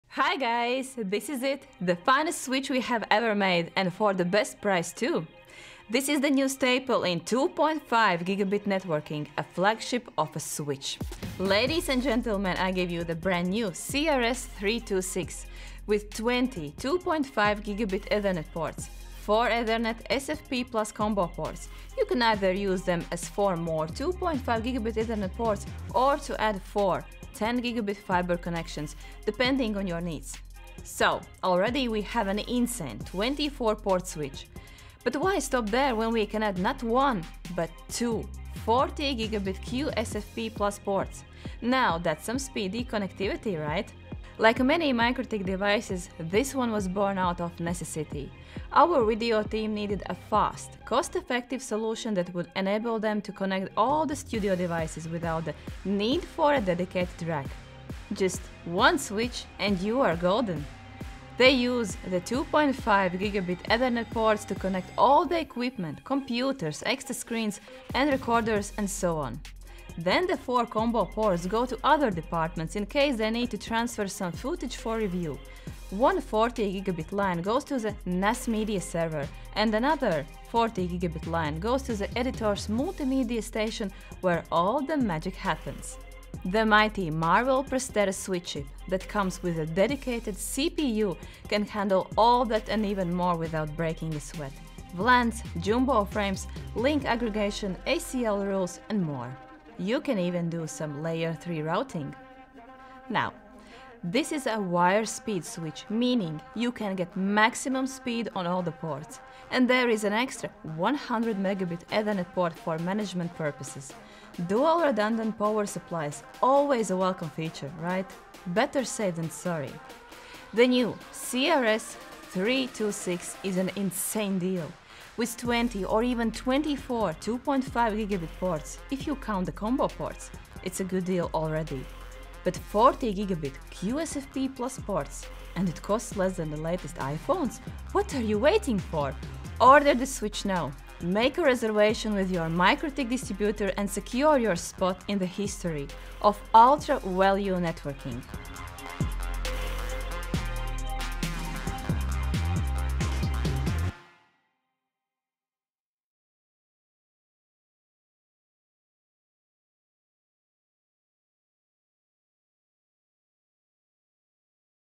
CRS326-4C+20G+2Q+RM: THE 2.5 Gigabit Ethernet switch!
Our top-of-the-line switch with incredible 2.5 Gigabit port density,
combo ports, and even a pair of 40 Gbps QSFP+ cages.
Enterprise-level gamechanger… for the price of a smartphone!